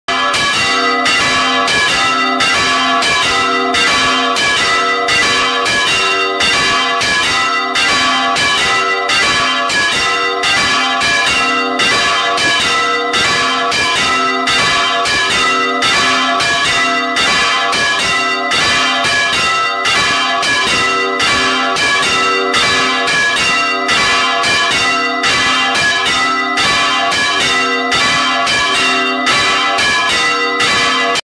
Ezcaray. Campanas de Nuestra Señora de Allende. part2
Volteo de las dos campanas mayores de la ermita de la Virgen de Allende el dia de su festividad. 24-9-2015